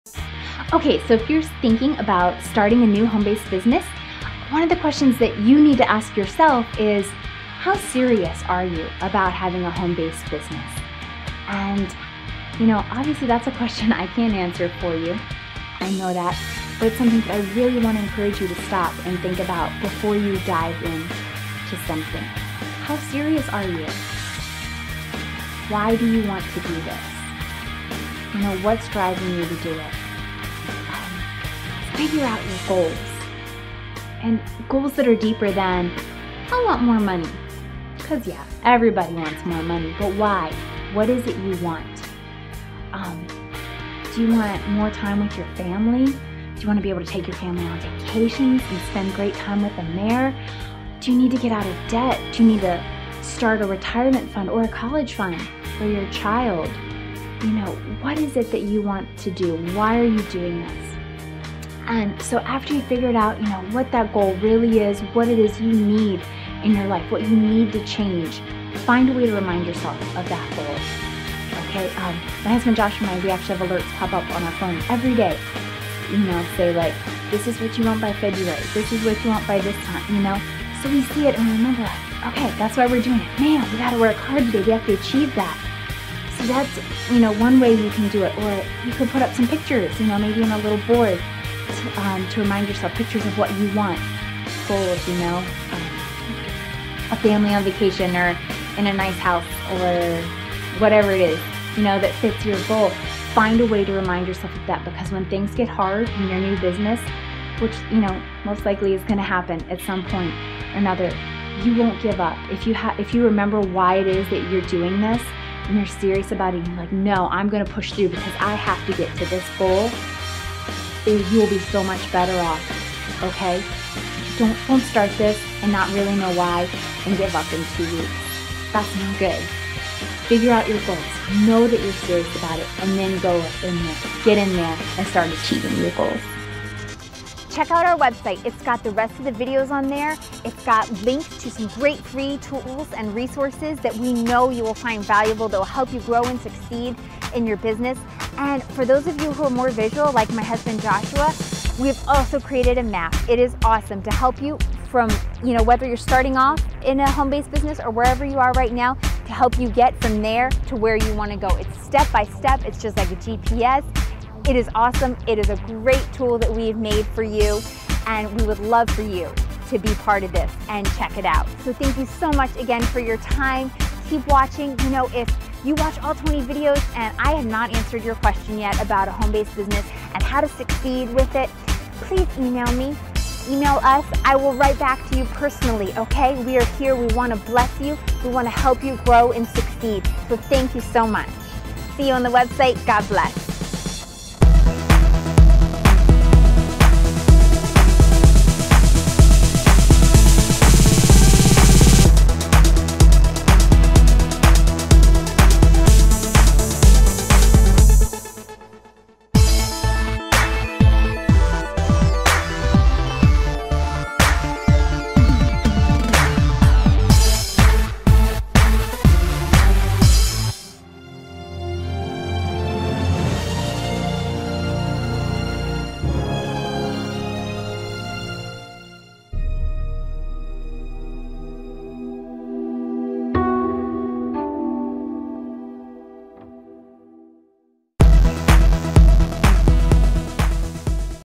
(1/20) Home Business Training: Get Serious About Success
We have learned a thing or two since joining a home business. It's time we shared the knowledge we've acquired and save people from failure and overwhelming heartache while getting a home business off the ground! Come join us and share your knowledge!

We made this video training series for Honest Solutions Network to lead you to the tools, trainings, and resources that you need to be successful with a home-based business. Our heart is to help you succeed and grow, without wasting your time, money, and energy on systems that don't work.